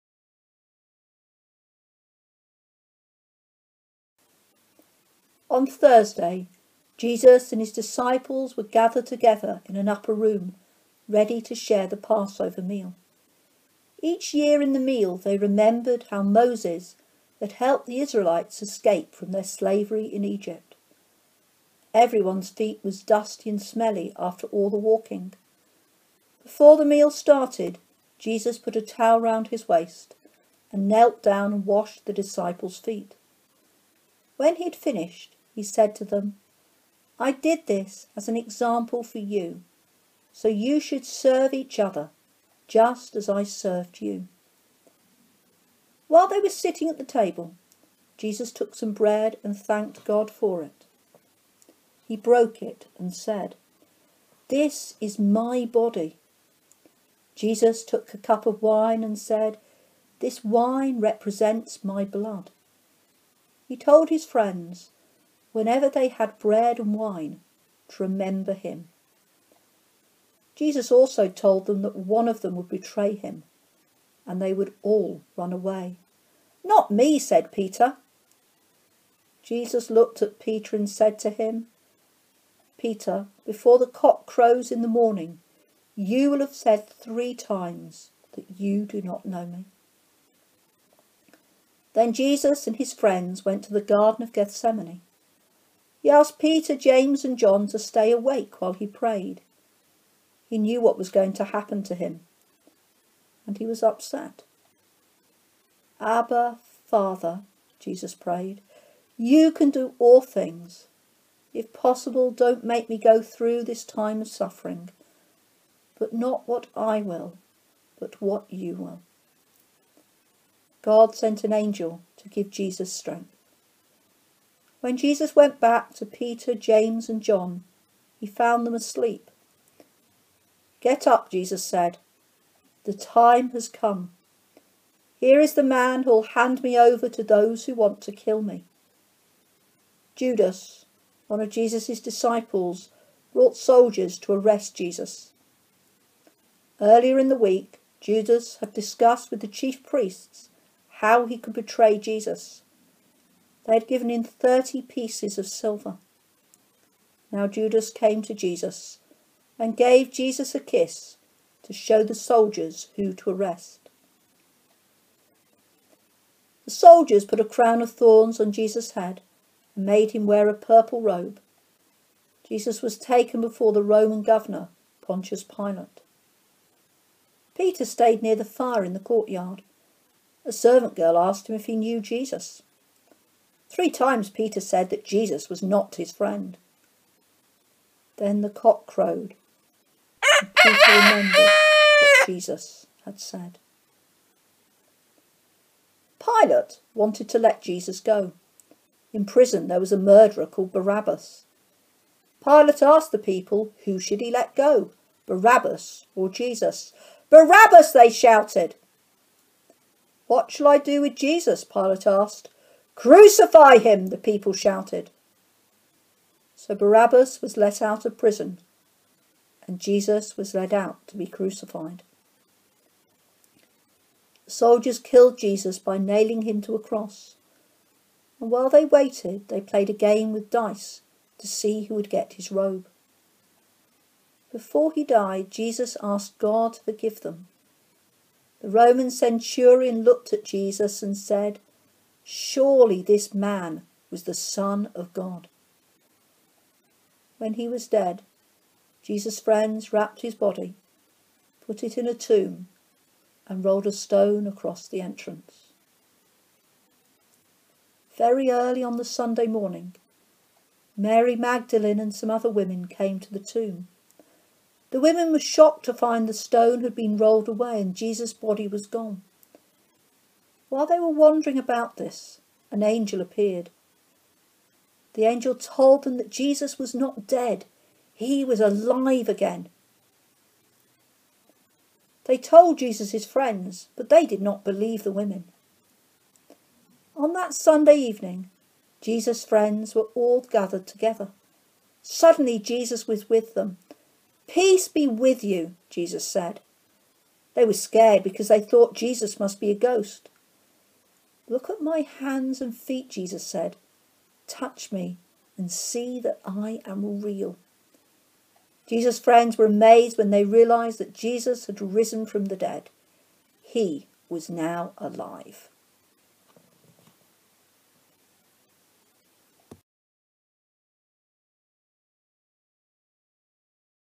The Story of Easter
Teddy Francis-John and friends present a dramatised version of the Bible story The Story of Easter